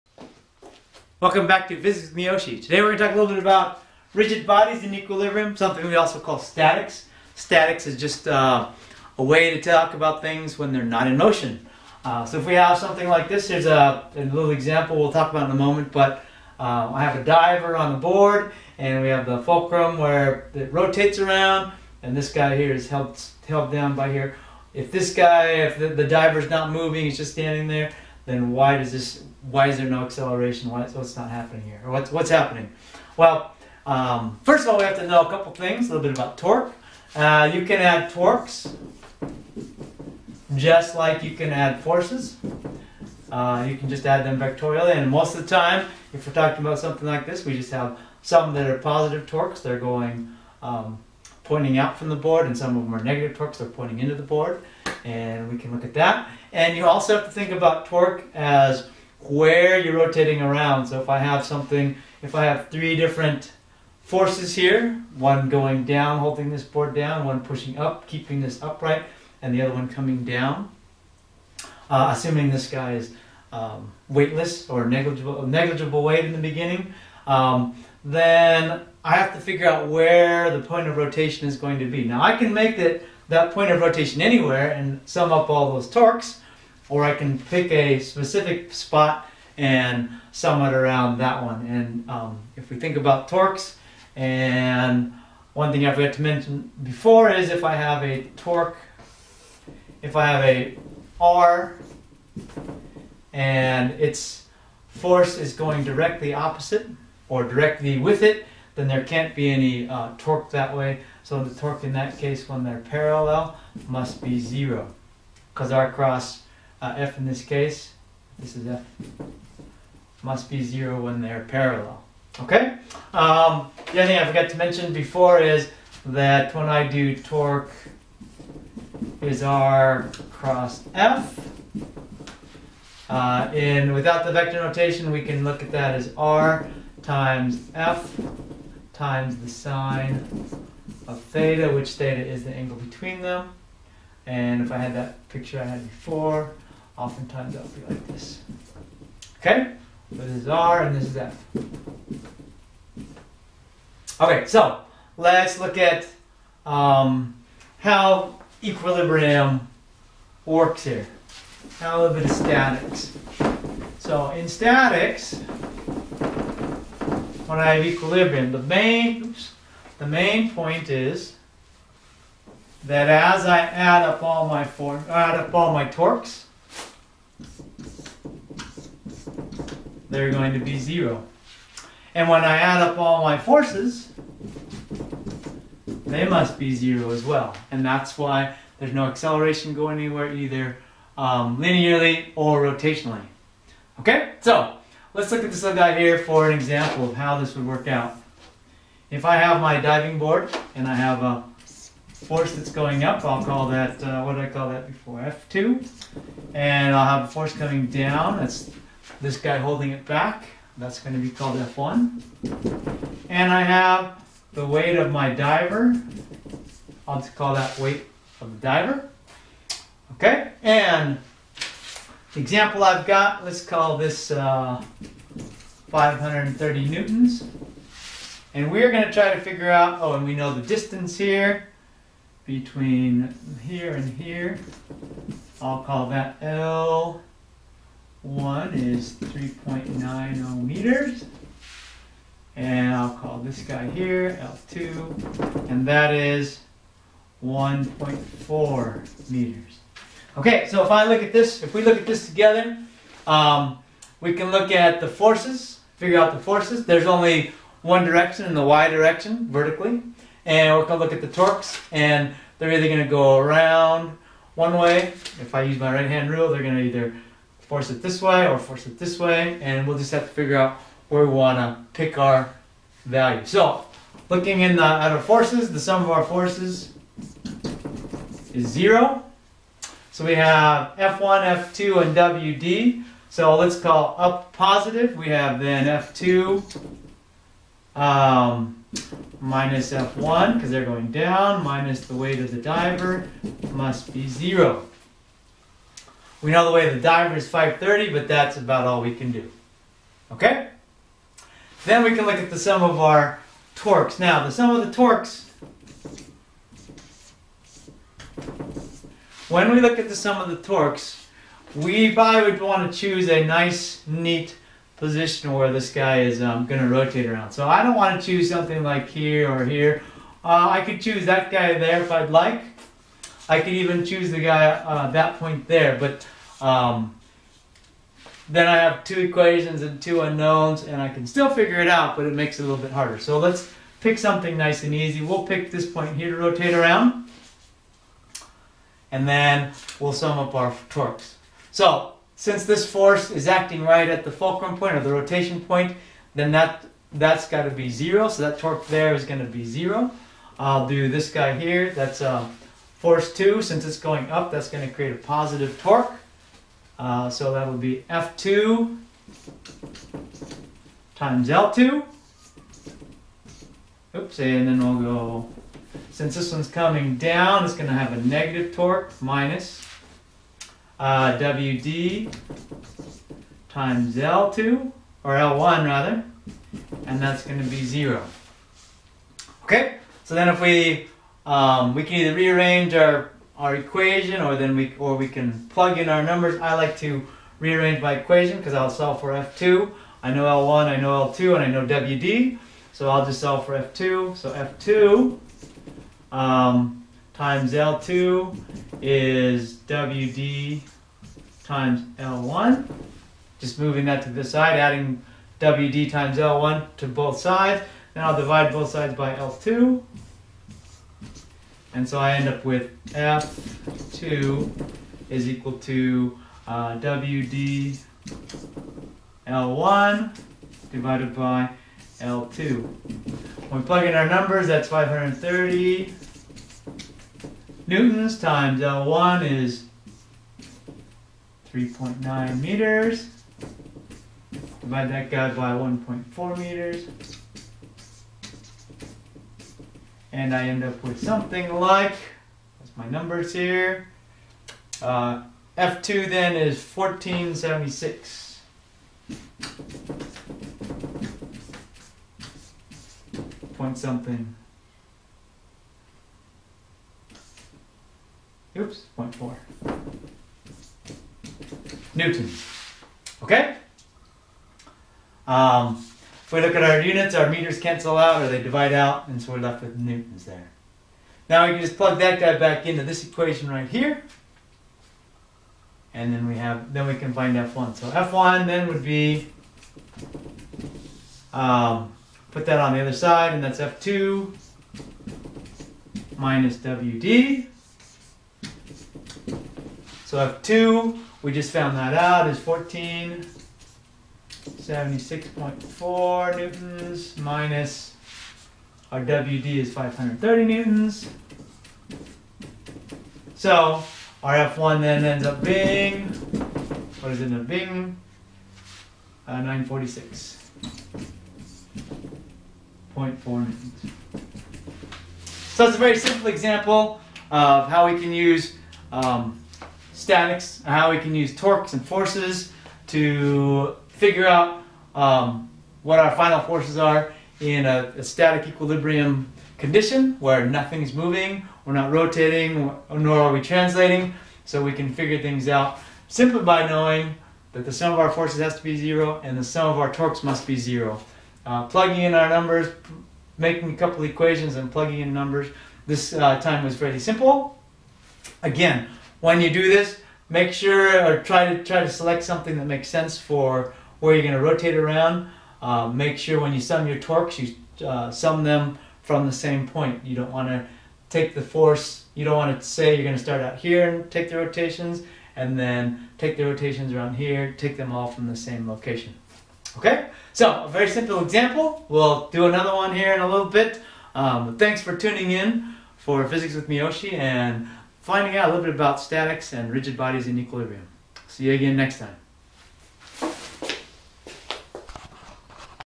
Physics with Miyoshi - 009-02-01 - Rigid Bodies in Equilibrium - Statics
Physics with Michael T. Miyoshi.  Rigid Bodies in Equilibrium.  Statics in one dimension.